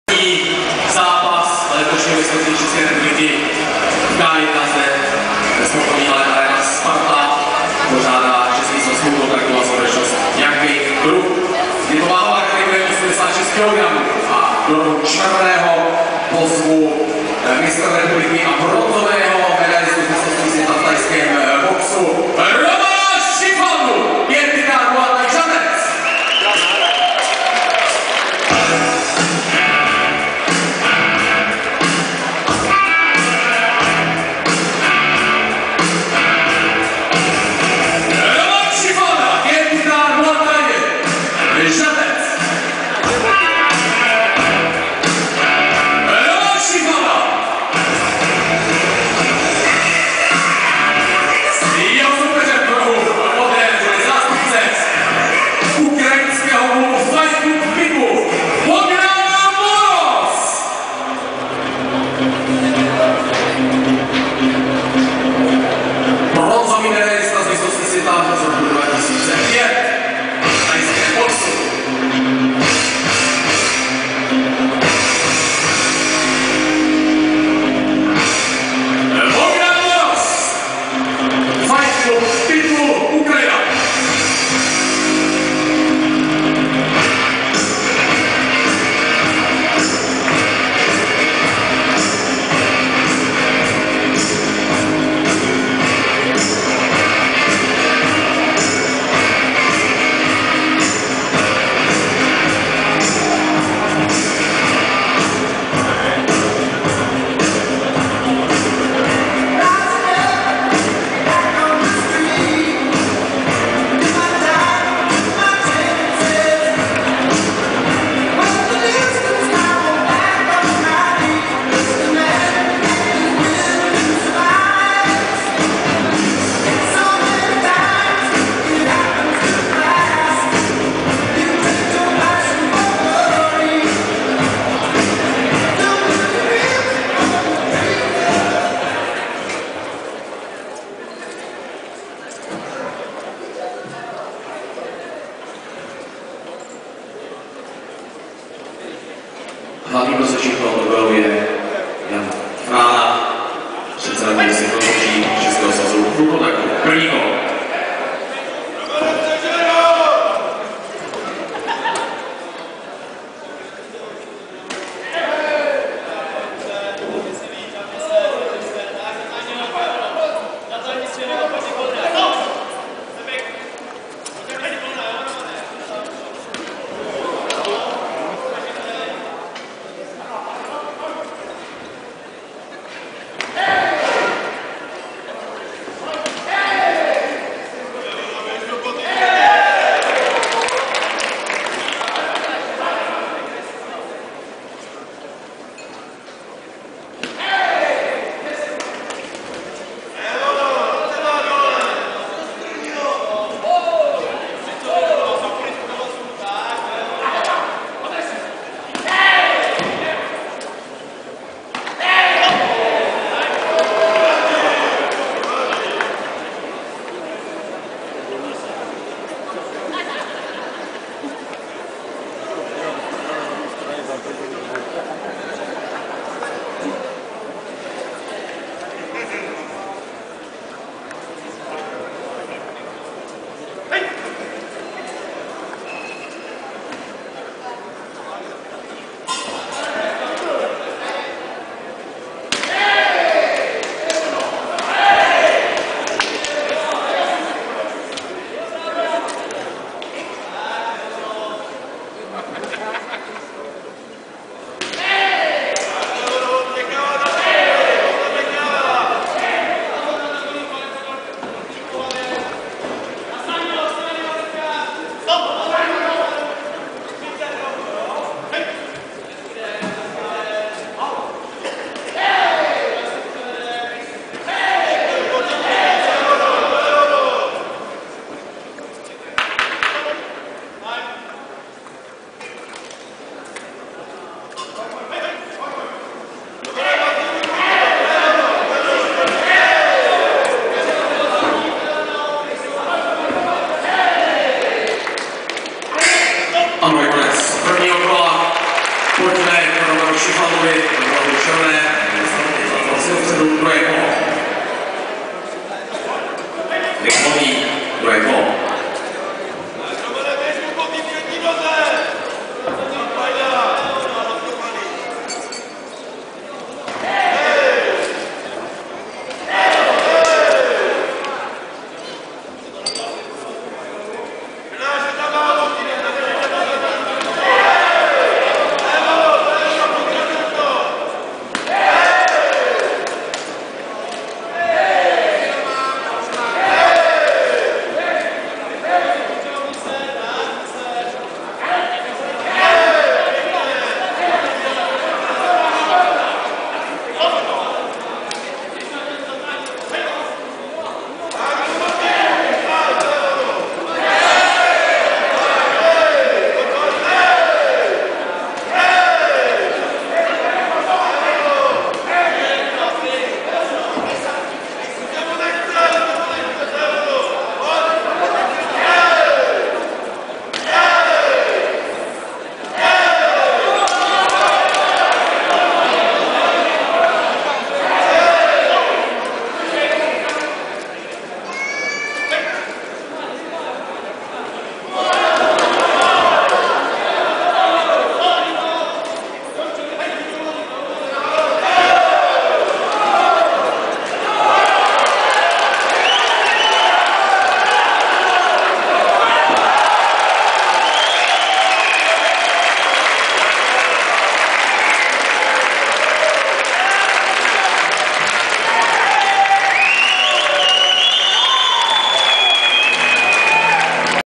Bogdan Moroz (UA) vs Roman Šifalda (CZ) 19.10.2013 -86kg Mistrovství ČR v K-1
Fight No. 9 - 86 Kg Men
RED: Roman Šifalda - G-Titan Žatec (CZ)
BLUE: Bogdan Moroz - Fightclub Pitbull (UA)